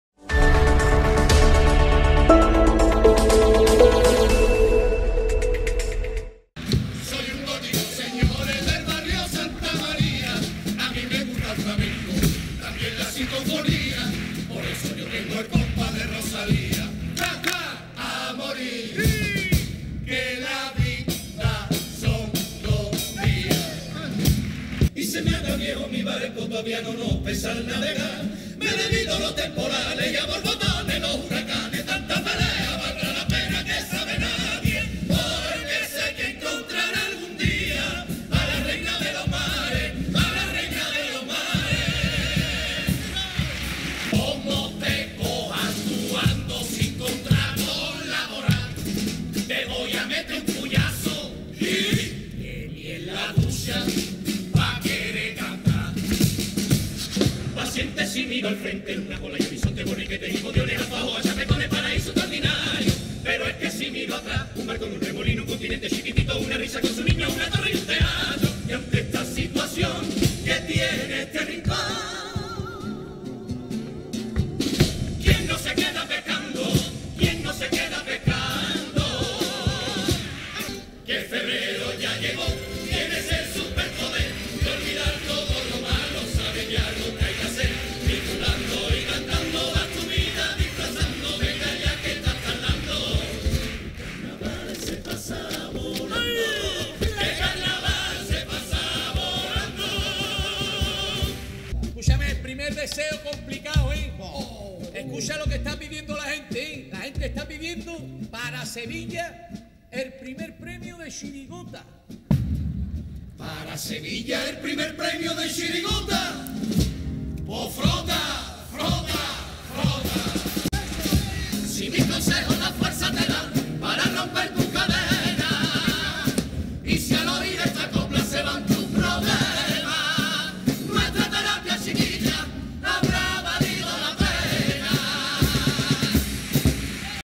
VÍDEO | Resumen de la Preliminar 15 del COAC 2020 (03/02/2020)
1ª .- Los niños de la calle Gotika (Chirigota)
2ª .- El viejo capitán (Comparsa)
3ª .- Los del puyazo bien dao..¡va por ustedes! (Chirigota)
4ª .- Los pacientes (Comparsa)
5ª .- Los superhombres (Coro)
6ª .- Los geni de Cádiz (Chirigota)
7ª .- La consulta (Comparsa)

Vídeo cedido por Onda Cádiz TV